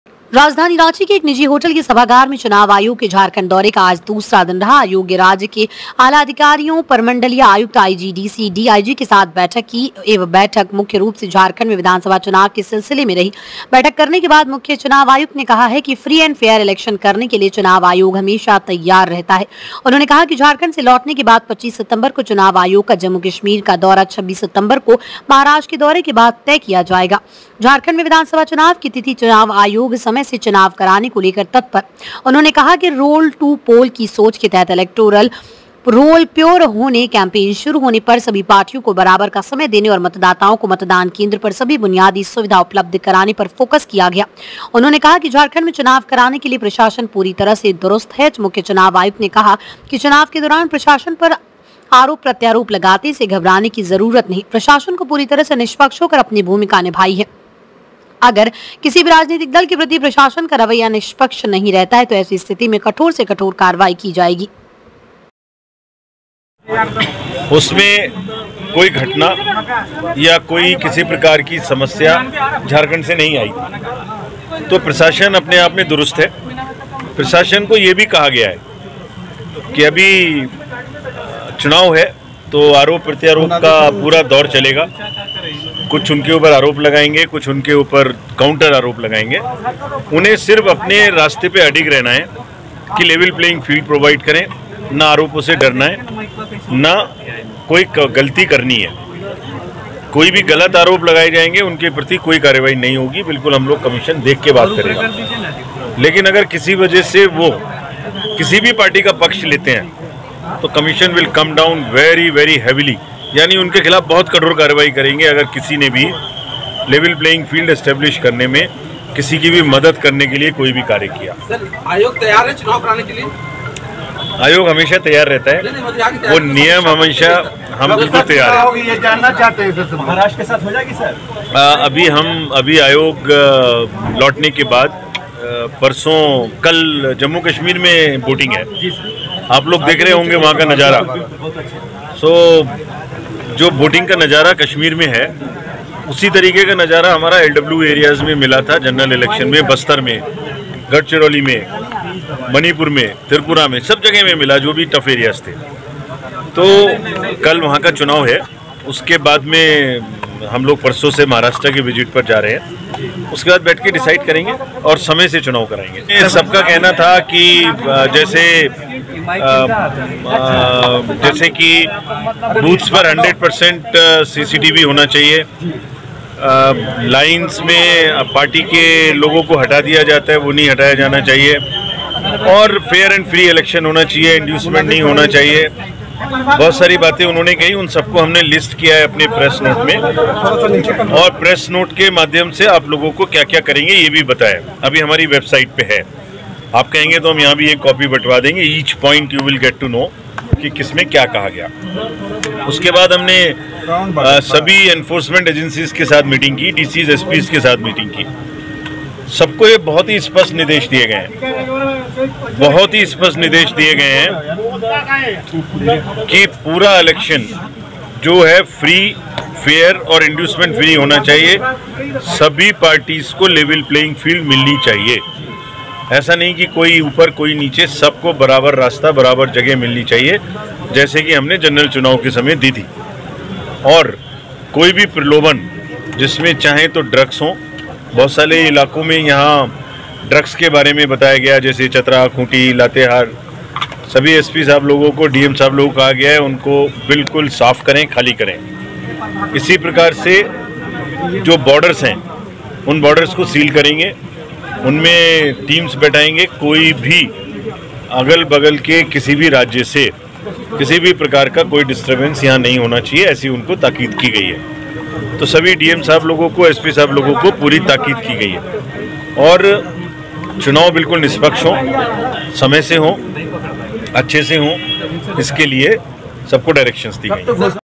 Ranchi || 26 सितंबर के बाद Jharkhand में चुनाव की तिथि की होगी घोषणा : मुख्य चुनाव आयुक्त
@SonaNewsIndia24Live  राँची

रिपोर्टर, चन्द्रप्रकाश रावत

हेडलाइन, 26 सितंबर के बाद झारखंड में चुनाव की तिथि की होगी घोषणा

हेडलाइन, चुनाव में प्रशासन पर आरोप प्रत्यारोप लगते हैं इससे घबराये नहीं :मुख्य चुनाव आयुक्त

हेडलाइन, निष्पक्ष होकर प्रशासन अपनी भूमिका निभाए : राजीव कुमार मुख्य चुनाव आयुक्त

एंकर, राजधानी रांची के एक निजी होटल के सभागार में चुनाव आयोग के झारखंड दौरे का आज दूसरा दिन रहा चुनाव
आयोग राज्य के आला अधिकारियों प्रमंडलीय आयुक्त आईजी डीसी डीआईजी  के साथ बैठक किये यह बैठक मुख्य रूप से झारखंड में विधानसभा चुनाव के सिलसिले में रहा
बैठक करने के बाद मुख्य चुनाव आयुक्त ने कहा कि फ्री एंड फेयर इलेक्शन करने के लिए चुनाव आयोग हमेशा तैयार रहता है। उन्होंने कहा की झारखंड से लौटने के बाद 25 सितंबर को चुनाव आयोग का जम्मू कश्मीर का दौरा, 26 सितंबर को महाराष्ट्र के दौरे के बाद तय की जाएगी झारखंड में विधानसभा चुनाव की तिथि चुनाव आयोग समय से चुनाव कराने को लेकर है तत्पर।
उन्होंने ने कहा कि *रोल टू पोल* की सोच के तहत इलेक्टोरक रोल प्योर होने, कैंपेन शुरू होने पर सभी पार्टियों को बराबर का समय देने और मतदाताओं को मतदान केंद्र पर सभी बुनियादी सुविधा उपलब्ध कराने पर फोकस किया गया है। उन्होंने कहा कि झारखंड में चुनाव कराने के लिए प्रशासन पूरी तरह से दुरुस्त है। मुख्य चुनाव आयुक्त ने कहा कि चुनाव के दौरान प्रशासन पर आरोप प्रत्यारोप लगाते हैं। इसे घबराने की जरूरत नहीं है। प्रशासन को पूरी तरह से निष्पक्ष होकर अपनी भूमिका निभाई है। अगर किसी भी राजनीतिक दल के प्रति प्रशासन का रवैया निष्पक्ष नहीं रहता है तो ऐसी स्थिति में कठोर से कठोर कार्रवाई की जाएगी।

Byte..: राजीव कुमार मुख्य चुनाव आयुक्त